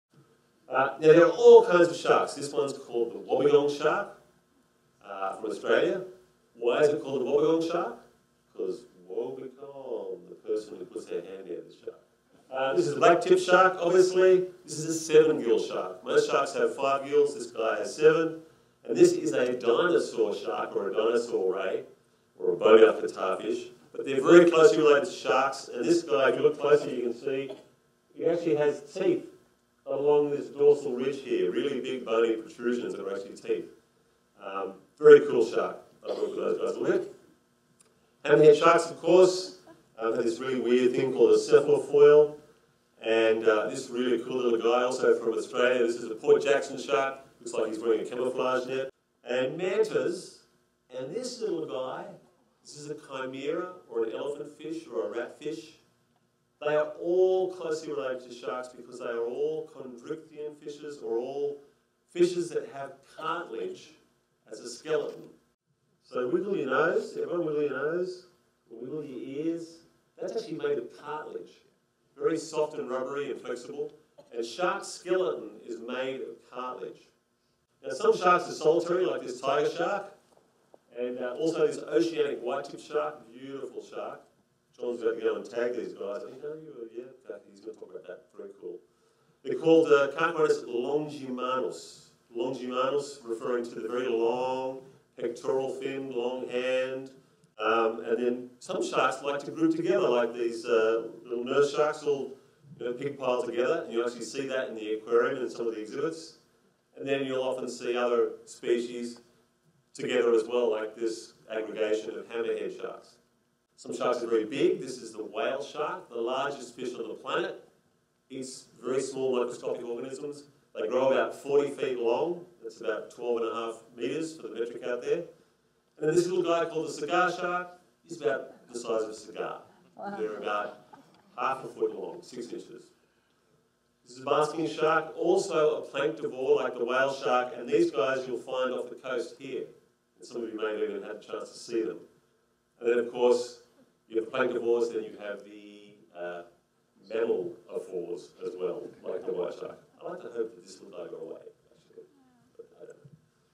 NEAQ's Mark Smith: So Many Kinds of Sharks!
To kick off a talk about the cultural impact, diversity and biology of sharks, New England Aquarium's VP Animal Care Mark Smith shows just how many kinds of sharks there are. To learn more about sharks and the NEAQ's marine conservation efforts, see the full lecture on  WGBH Forum Network:

Explore a world of ideas